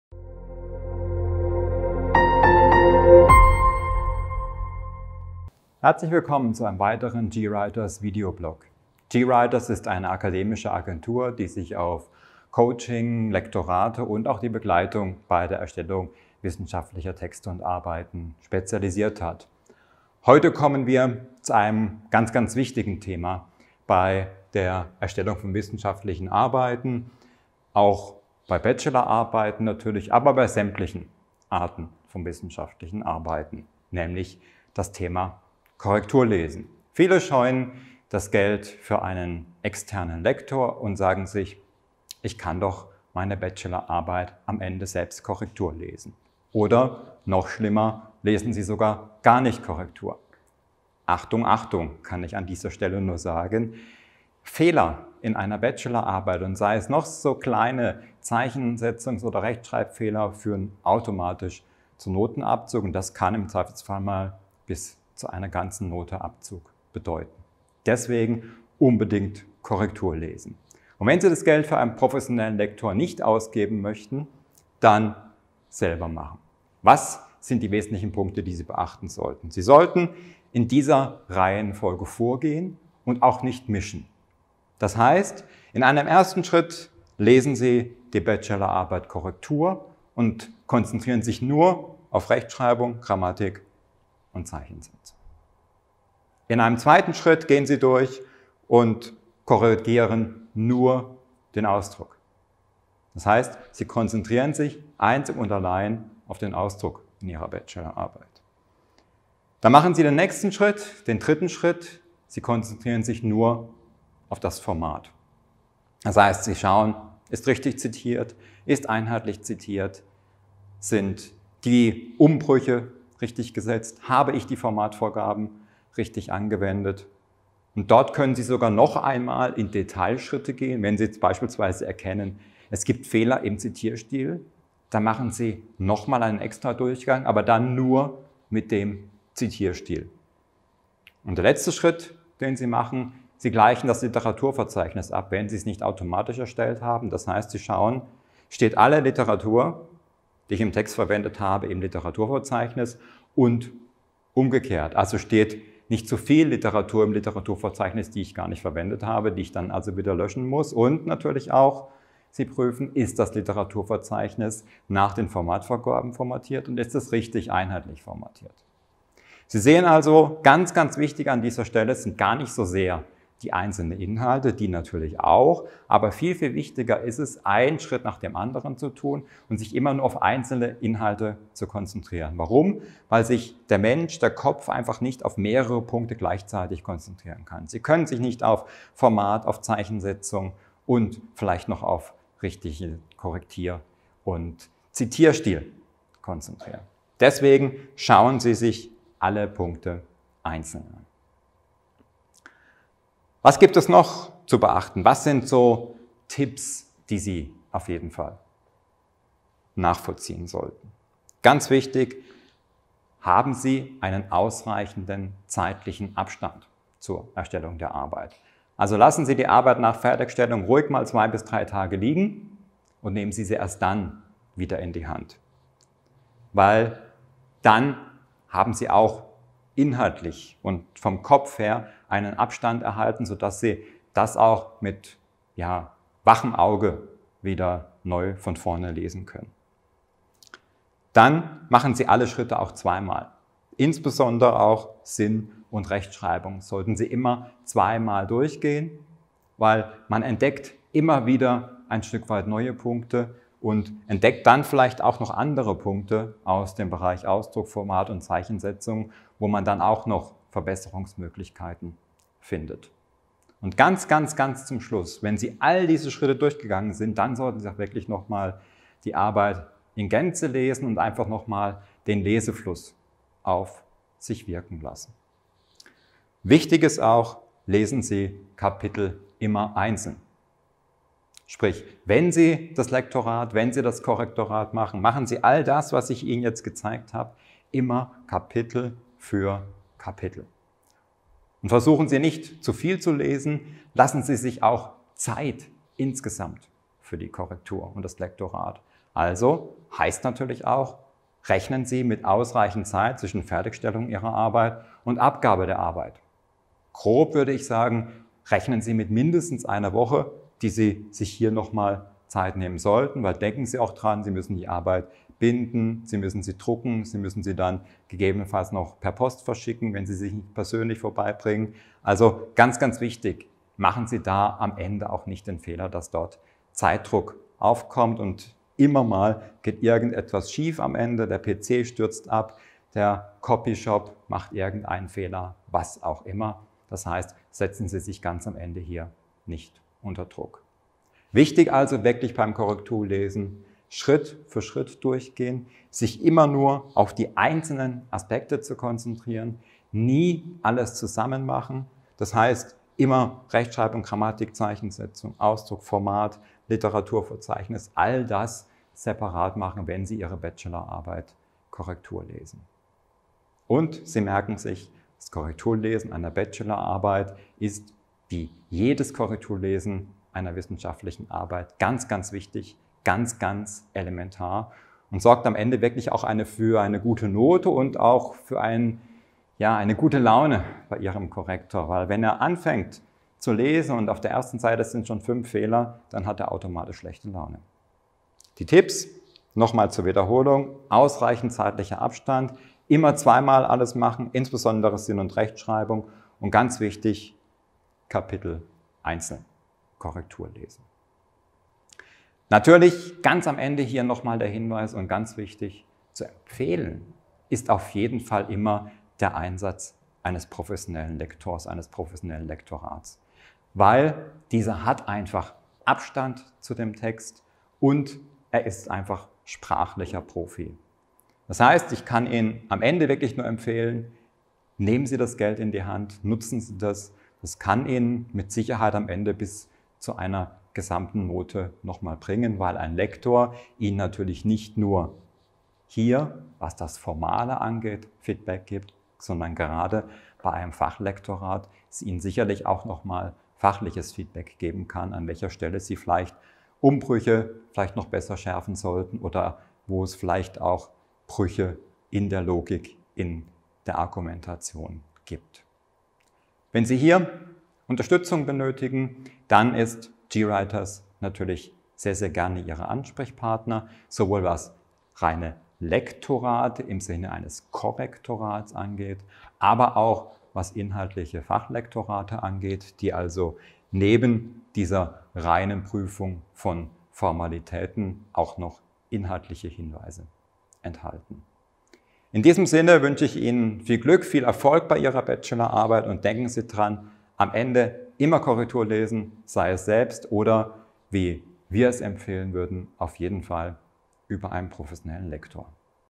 Bachelorarbeit Кorrektur lesen - Sicher zum Abschluss!
GWriters • • Dass die Bachelorarbeit vor der Abgabe korrigiert werden muss, ist jedem Studierenden bekannt. Die Theorie erfolgreich in die Tat umzusetzen aber, fällt vielen schwer. In unserem neuesten Videobeitrag geben wir Ihnen unsere Expertentipps, wie Sie selber bei Ihrer Bachelorarbeit Korrektur lesen können und wann Sie Ihre Bachelorarbeit Korrektur lesen lassen sollten.